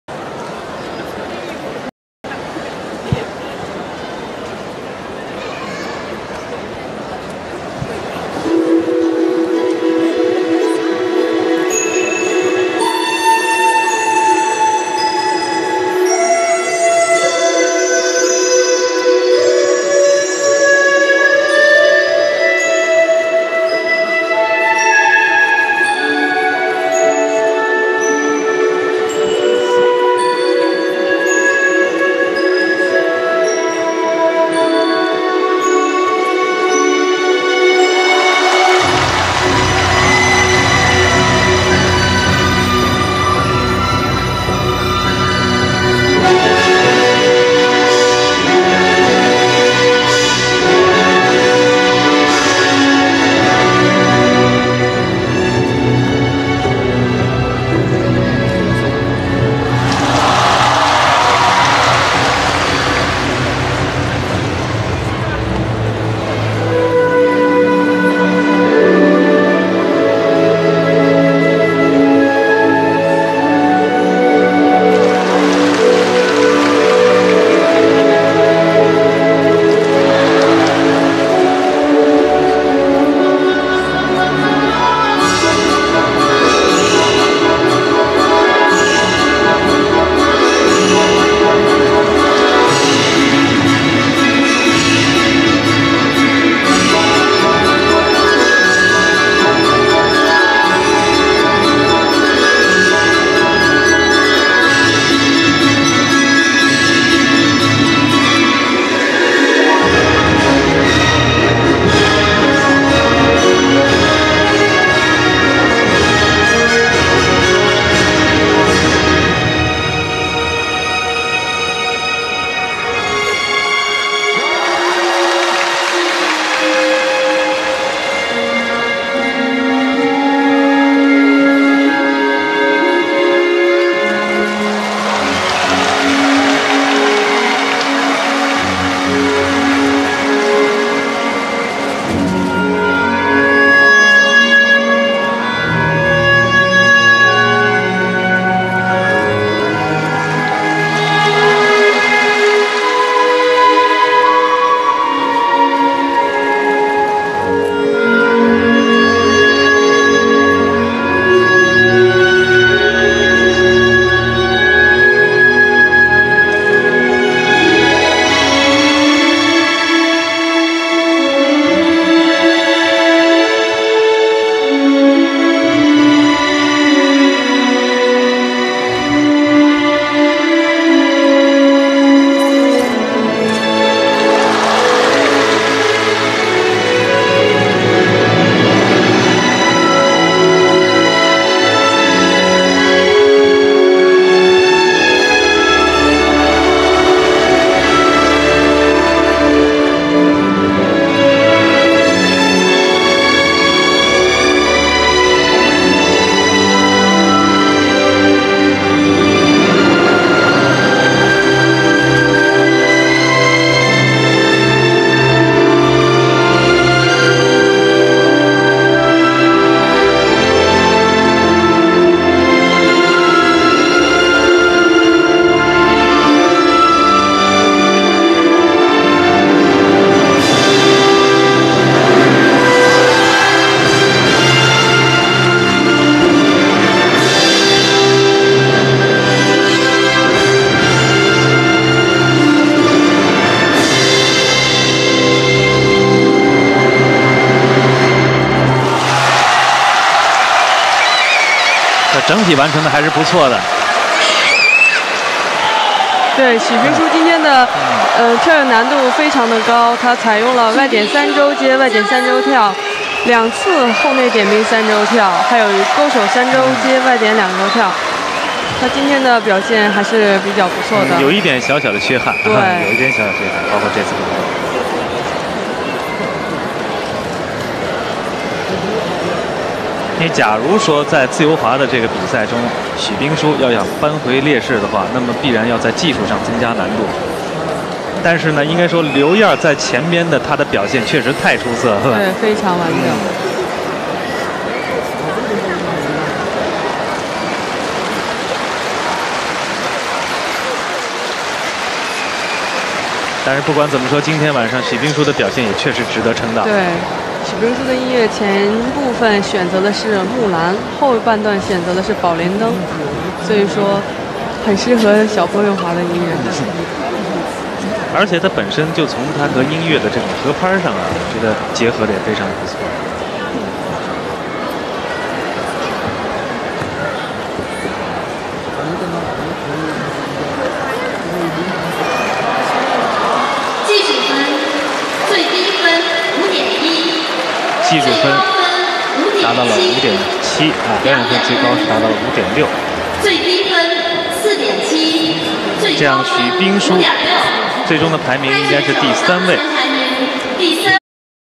Binshu Xu 05Nationals LP
Binshu Xu 05 Nationals LP,placed 3rd after free skate.Won the bronze medelist in Nanjing,where 06 COC will be held.